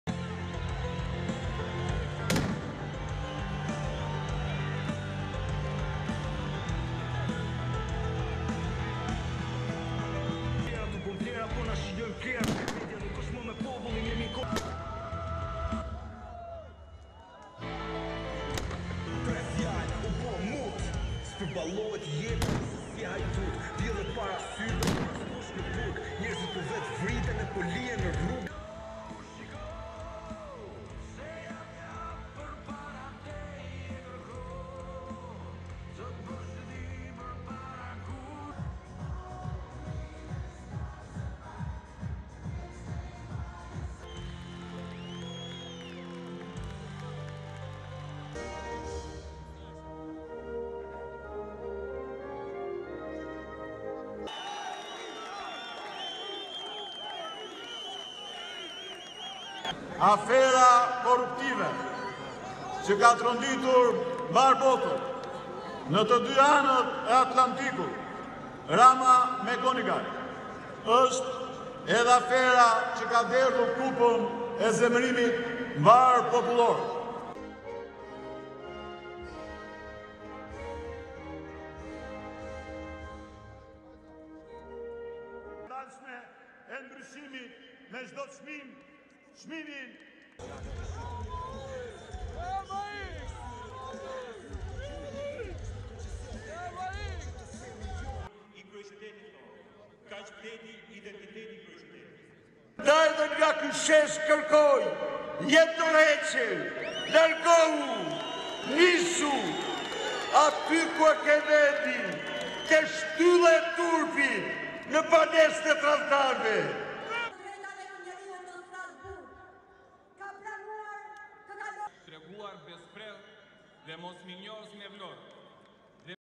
Albania opposition leads anti-government protest
RESTRICTION SUMMARY:

ASSOCIATED PRESS
Tirana - 11 February 2023
1. Various of smoke bombs thrown by opposition supporters at the end of the protest, police officers wiping out the smoke bombs
2. Anti-riot police ready to intervene
3. Wide of opposition supporters at the rally
4. Protesters holding Prime Minister Edi Rama’s photo and that of US Ambassador to Tirana Yuri Kim, alongside banner which read (Albanian) “When Berisha (Former Prime Minister Sali Berisha) is declared persona non grata by the USA, when the USA investigates (former FBI agent Charles) McGonigal’s scandal”
5. Wide of protesters, a banner reads (Albanian) “We own the ballot” and “We own pluralism”
6. SOUNDBITE (Albanian) Ilir Meta, Freedom Party:
“The corruptive affair which has shaken worldwide, on both coasts of the Atlantic, Rama-McGonigal is the affair which has sparked a nationwide anger.”
7. Protesters holding a banner which reads (Albanian) “What you can suffer from the FBI, God can hardly do”
8. A protester holding a mask with writing which reads (Albanian) “Don’t stay silent”
9. Sali Berisha of the Democratic Party heading to the rally, accompanied by other leaders and bodyguards
10. Big poster with photos of Rama and Kim which reads (Albanian) “Yuri Kim hailing my fight against corruption” and “Me who has corrupted a former FBI head”
11. SOUNDBITE (Albanian) Sali Berisha, Democratic Party:
“From this square I ask you, resign, go away, go to where you belong, at the pillar of shame, at the traitors’ shelter”
12. Wide of protesters outside the government’s building
13. Wide of the protesters with banners and US flags
STORYLINE:
Thousands of supporters of Albania's political opposition took part in an anti-government protest Saturday, calling for the prime minister's resignation for alleged corruption and mishandling of the small Balkan nation's economy.

Sali Berisha, a former president and prime minister of Albania who leads the center-right Democratic Party, and former President Ilir Meta, who leads the left-wing Freedom Party, were among the protesters who gathered in front of the main government building with banners and shouted for Prime Minister Edi Rama of the ruling center-left Socialist Party to resign.

“From this square I ask you, resign, go away, go to where you belong, at the pillar of shame, at the traitors’ shelter”, he said.

Protest participants hurled smoke bombs at the main entrance of the government building, where hundreds of police officers formed a line to protect the building.

Rama's critics accused him of corruption and hold him responsible him for the exodus of young Albanians who have left to pursue jobs in Western European countries.

Rama says his Cabinet has kept inflation low compared to elsewhere in Europe, and has subsidized electricity for families and small businesses.

The opposition also accuses Rama of corrupting Charles McGonigal, a former high-ranking FBI counterintelligence official, who is accused in the United States of hiding from the FBI key details of a 2017 trip he took to Albania with a former Albanian intelligence official who is alleged to have given him at least $225,000.

McGonigal met with Albania’s prime minister and urged caution in awarding oil field drilling licenses in the country to Russian front companies.

Rama has acknowledged meeting with McGonigal but denied allegations of giving him money or preferential treatment.

Britain did the same last year.

Berisha called for another protest at the parliament on Monday.

Albania is to hold municipal elections in May.

AP Video by Erion Xhabafti